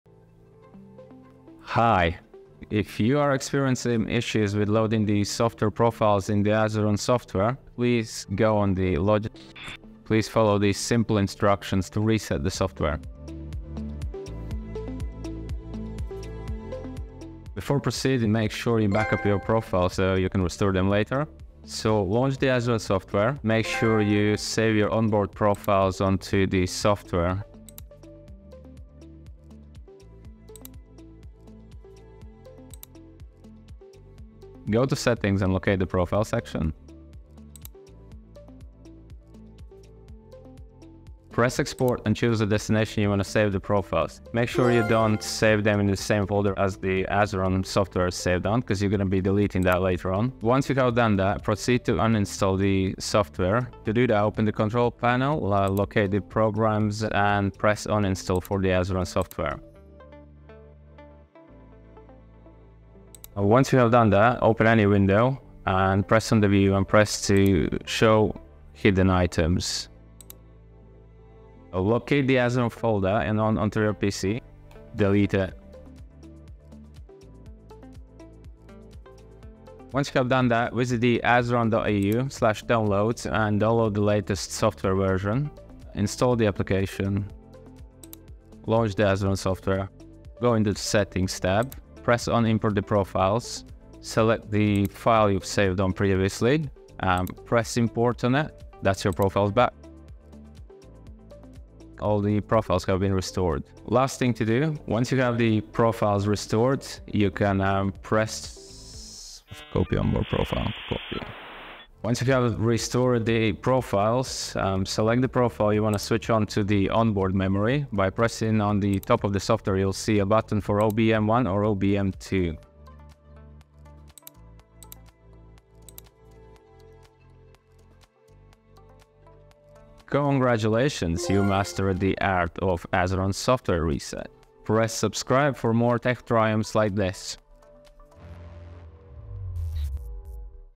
Master the art of Azeron Software reset
Follow this simple guide to fully reset your Azeron software.